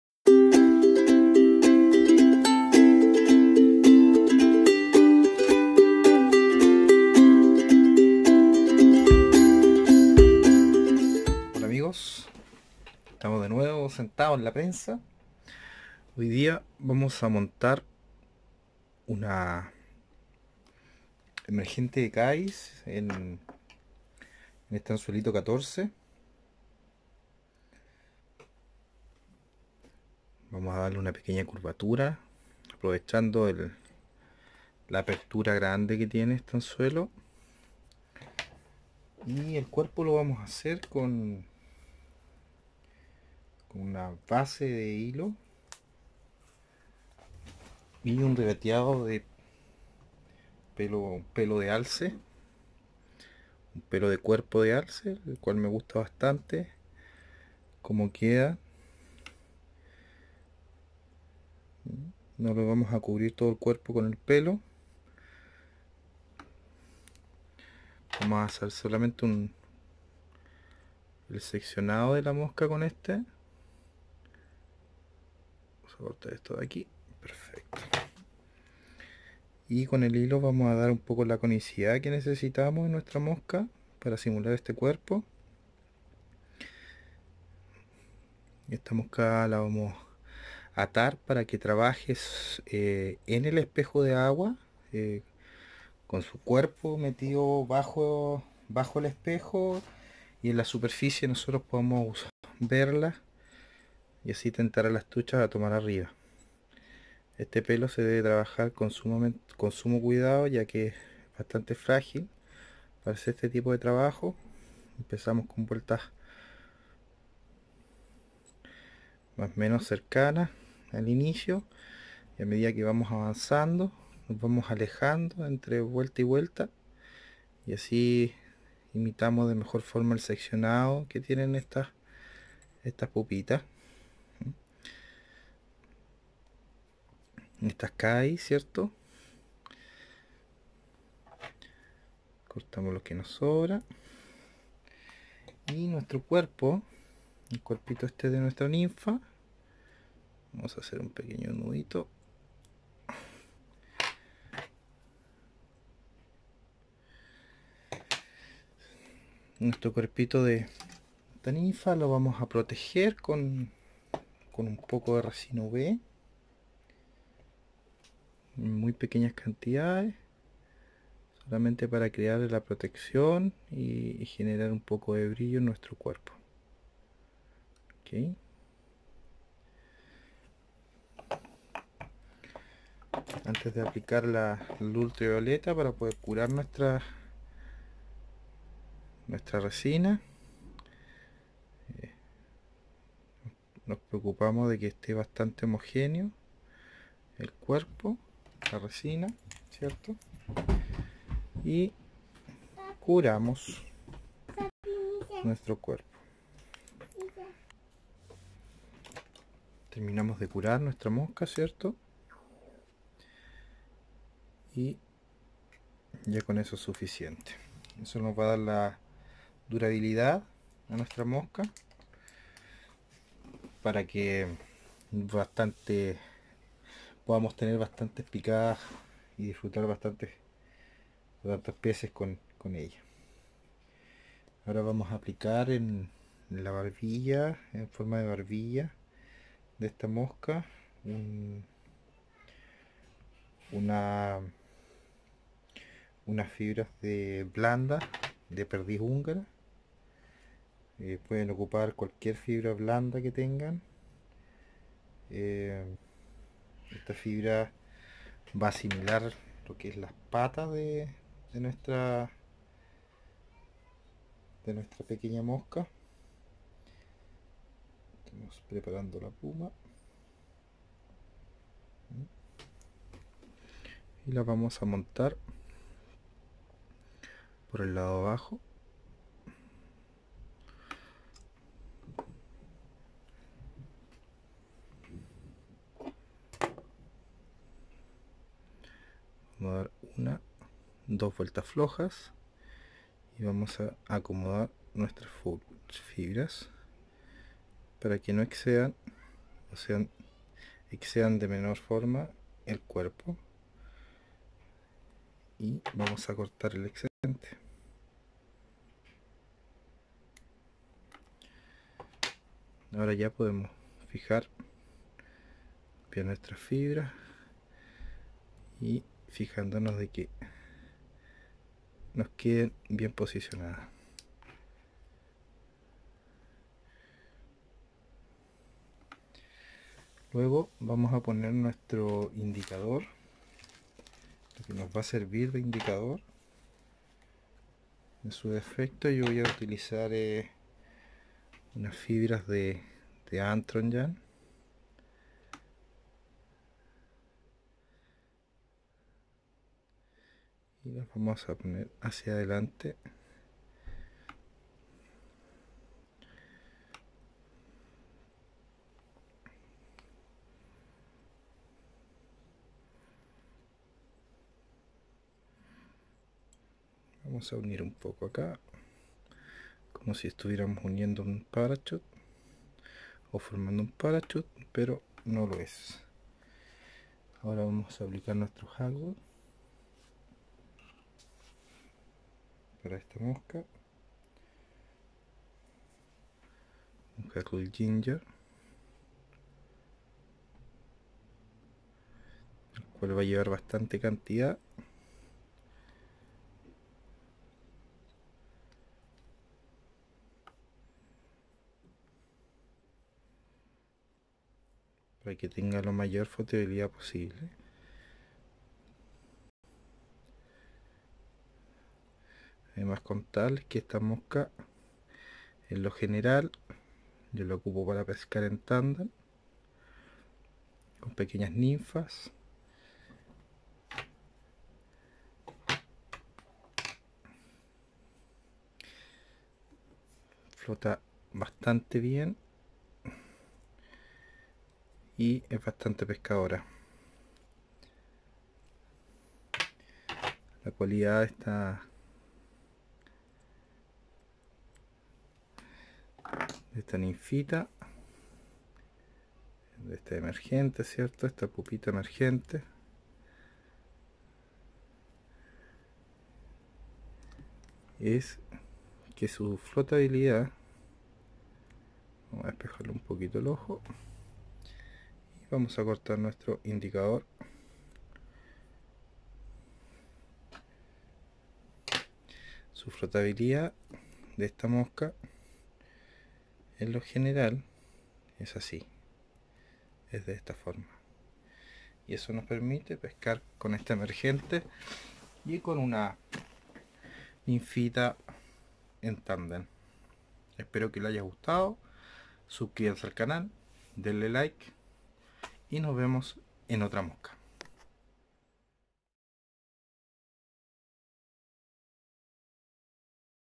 Emergente de caddis
Emergente que no puede faltar en tu caja, simple y efectiva la puedes usar como tanden o bien sola, esta mosca no te dejará quedar mar ante una eclocion de tricoptero...